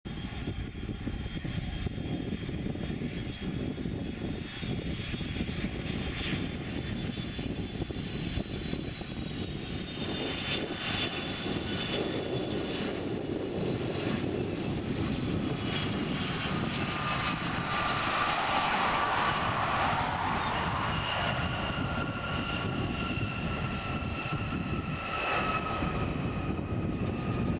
[HD] Delta Airlines Boeing 757-2Q8 Philadelphia Intl Airport
May 5, 2012 | Philadelphia Intl Airport (KPHL/PHL)
Delta Airlines | Boeing 757-2Q8
Tail #: N709TW

NOTE: I realize the video is bumpy, this was my first time spotting with this camera and I am still figuring it out.